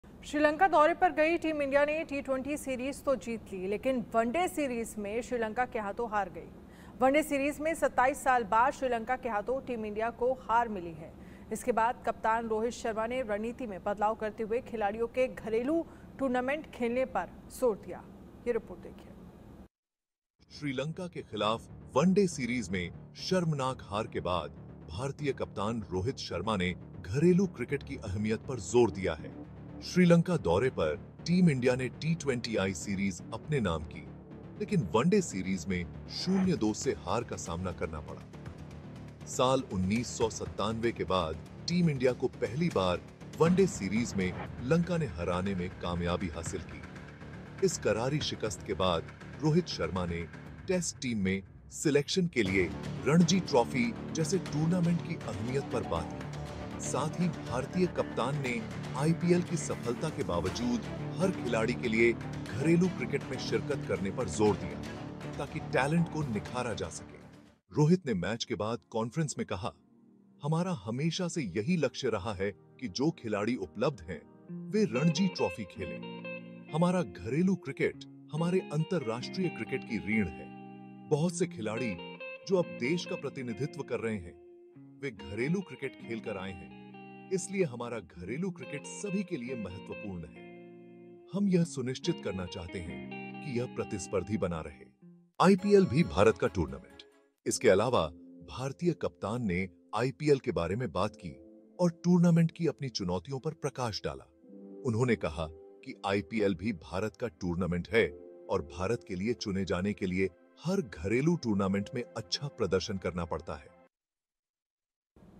ODI Series: श्रीलंका से वनडे सीरीज हारने के बाद रोहित शर्मा को आया गुस्सा कह दी बड़ी बात! |India News
टीम इंडिया के कप्तान रोहित शर्मा ने कहा है कि टेस्ट और वनडे के लिए टीम के चयन के लिए रणजी ट्रॉफी जैसे घरेलू टूर्नामेंट काफी अहमियत रखते हैं। उन्होंने माना कि चयन के लिए आईपीएल भी एक अहम टूर्नामेंट है, लेकिन उन्होंने कहा कि इसके आने से भारत की प्रमुख घरेलू प्रतियोगिताओं का महत्व कम नहीं हुआ है। बता दें, श्रीलंका के खिलाफ तीन मैच की वनडे सीरीज में भारत को 2-0 से हार का सामना करना पड़ा, सीरीज का पहला मैच टाई रहा था। भारत 1997 के बाद श्रीलंका से वनडे सीरीज हारा है। इस मैच में श्रीलंका की धीमी पिचों पर भारतीय बल्लेबाज संघर्ष करते नजर आए।

Team India captain Rohit Sharma has said that domestic tournaments like Ranji Trophy are very important for team selection for Test and ODI. He admitted that IPL is also an important tournament for selection, but he said that its arrival has not reduced the importance of India's major domestic competitions. Let us tell you, India had to face defeat 2-0 in the three-match ODI series against Sri Lanka, the first match of the series was a tie. India has lost the ODI series to Sri Lanka after 1997. In this match, Indian batsmen were seen struggling on the slow pitches of Sri Lanka.